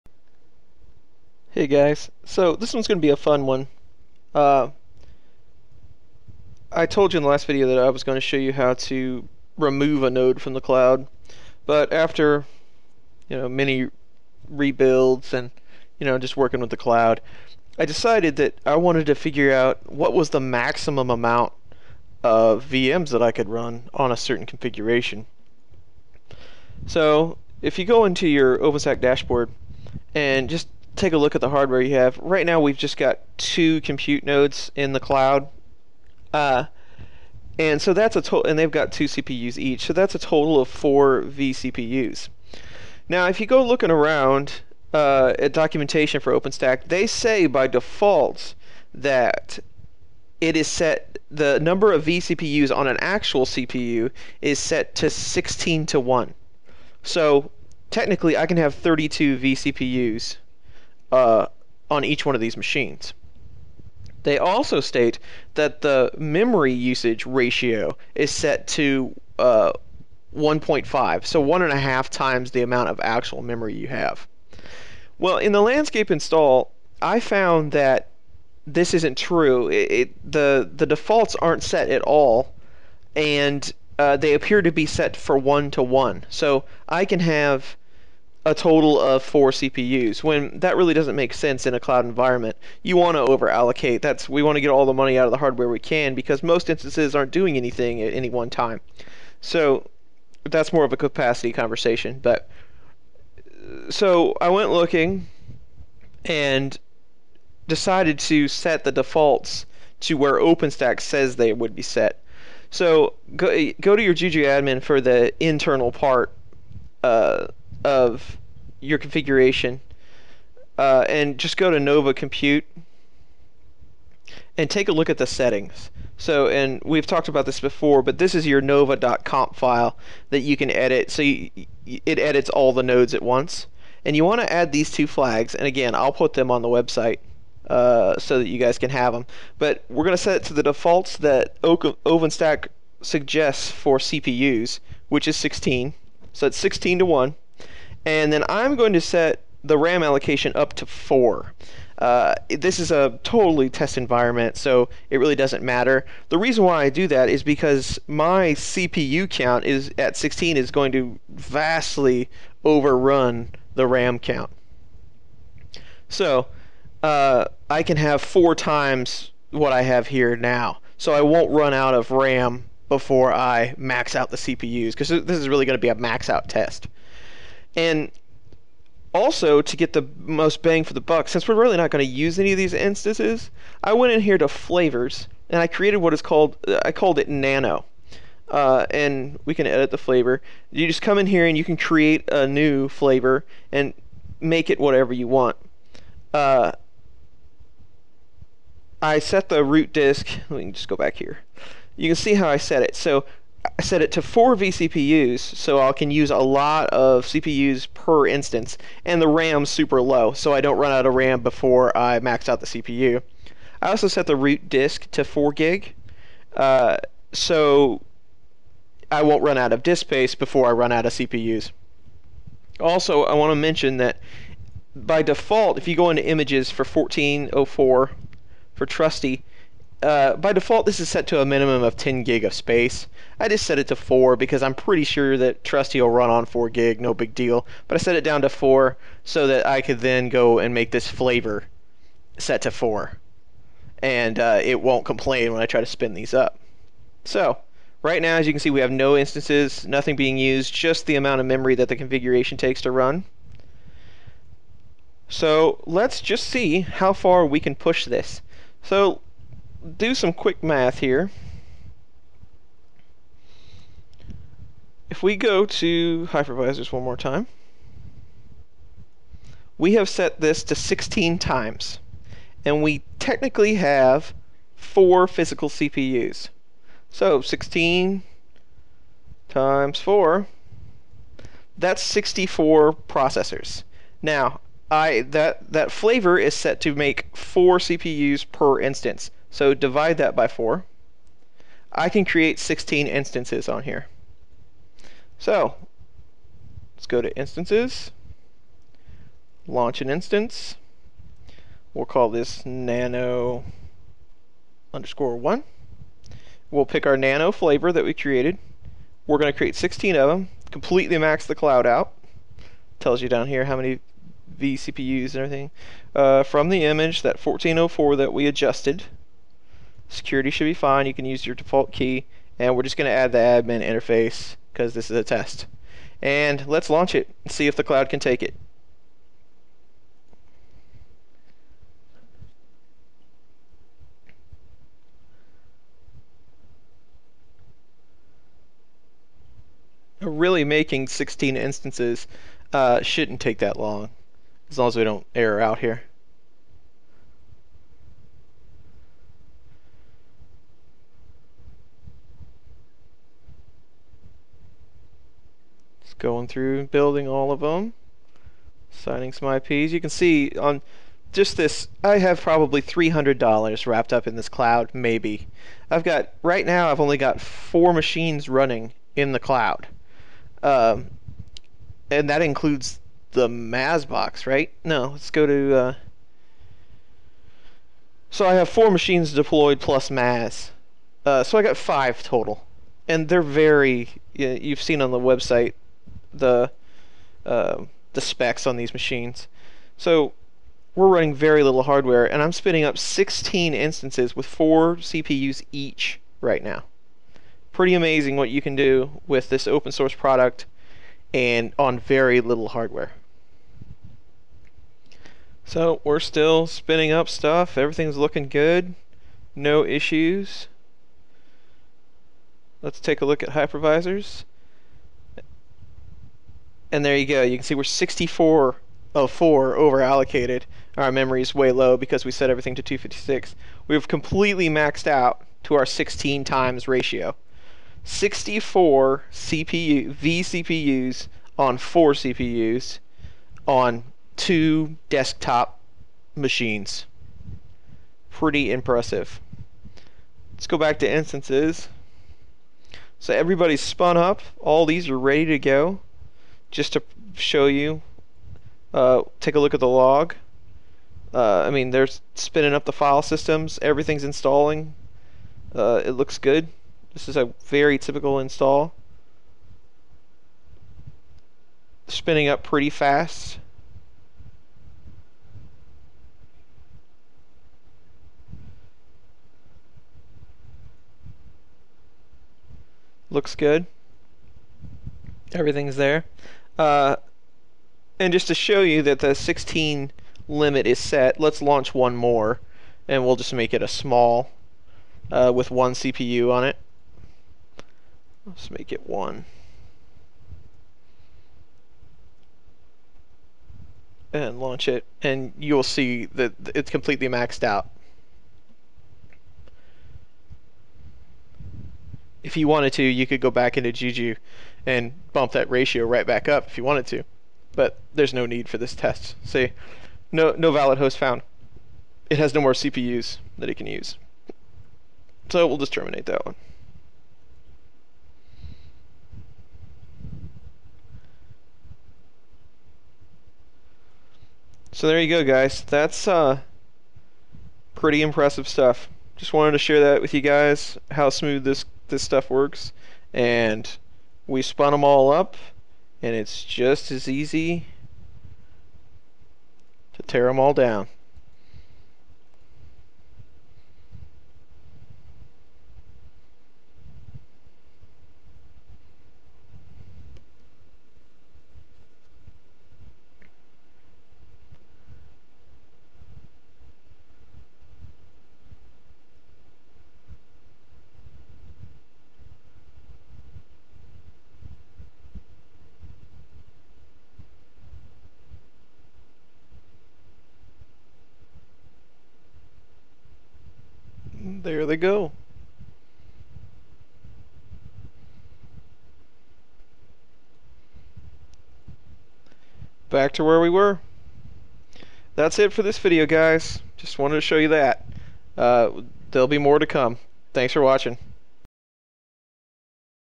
Openstack Cloud 16 4 vCPU instances on 4 CPUs two computers
In this video I show how with very little you can spin up 64 vCPUs or more on just 2 machines. Very cool stuff!